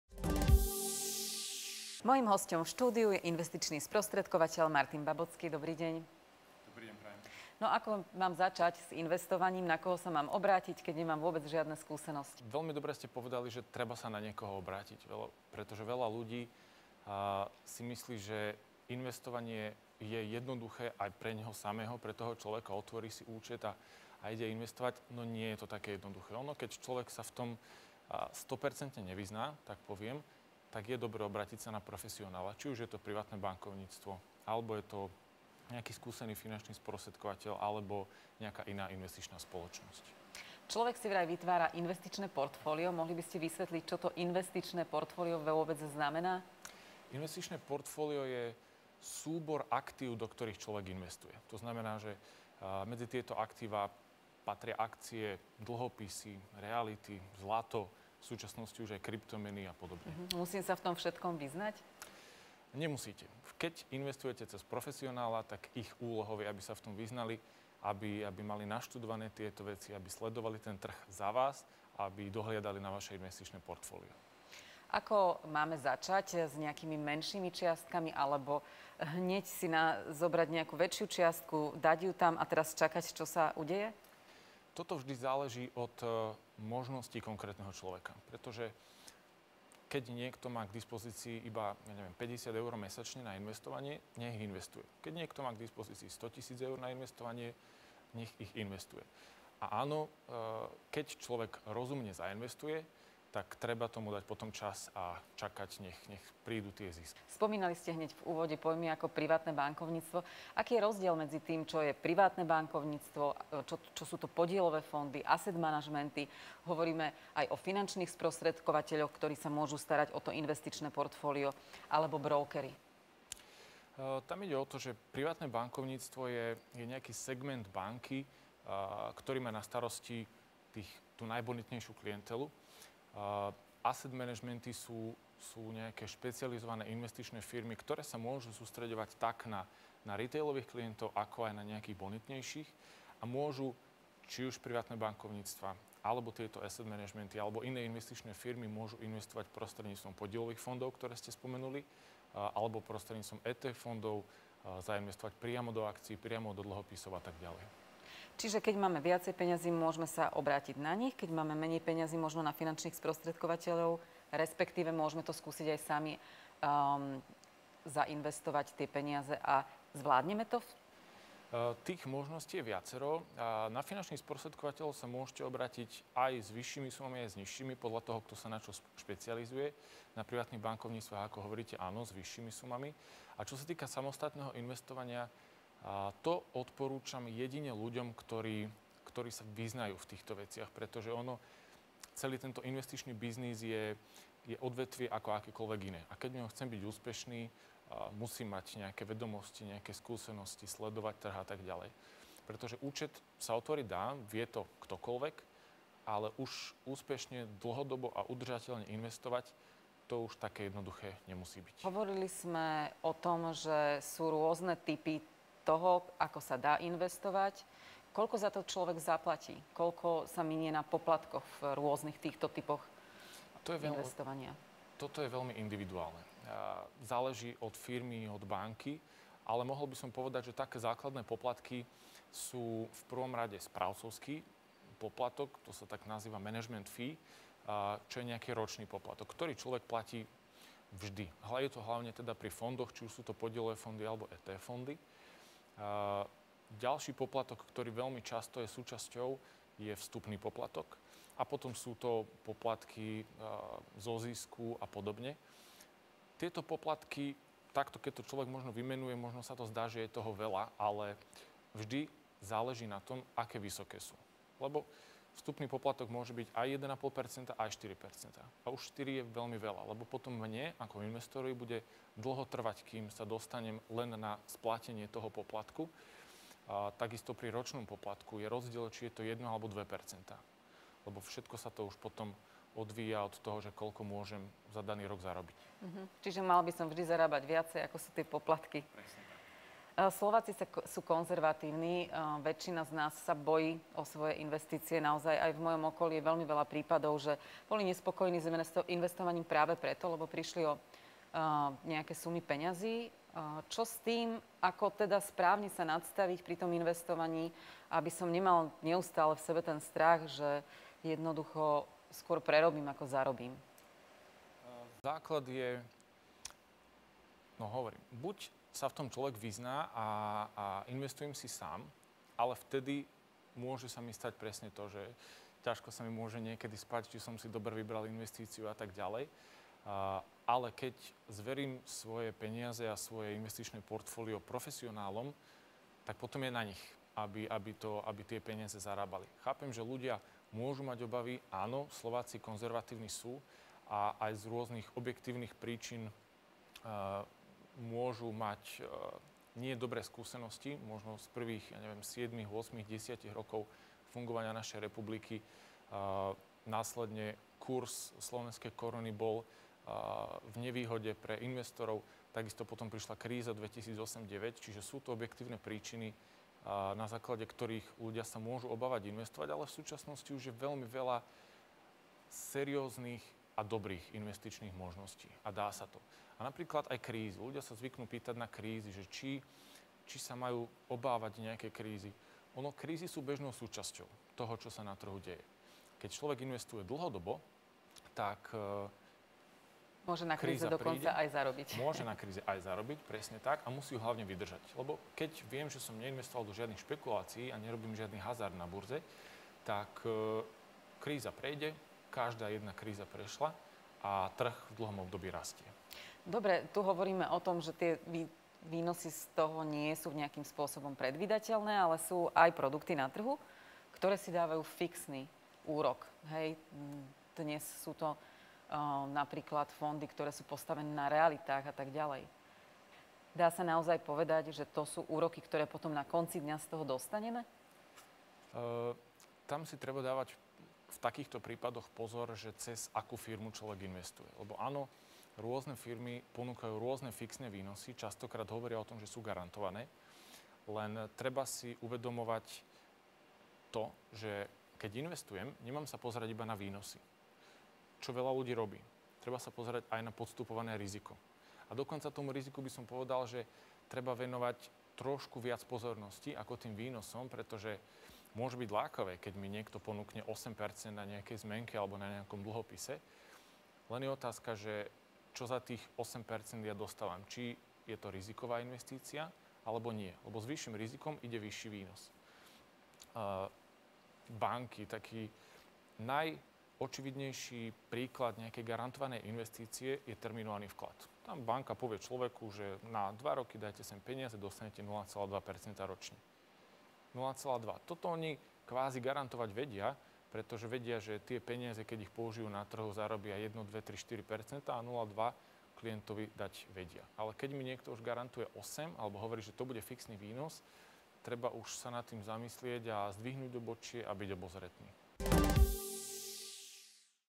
Základy investovania podľa investičného sprostredkovateľa Martina Babockého
Investičný sprostredkovateľ Martin Babocký nám v poslednom dieli relácie povedal o základoch investovania.